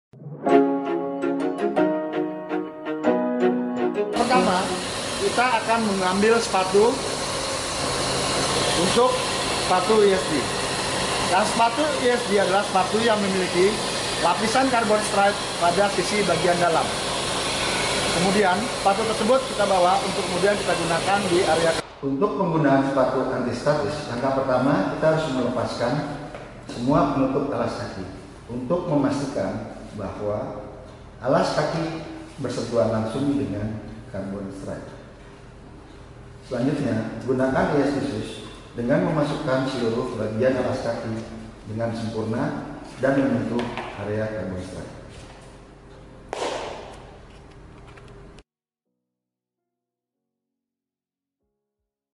Pemakaian Sepatu Antistatis
Video ini menjelaskan  pemakaian sepatu antistatis dengan benar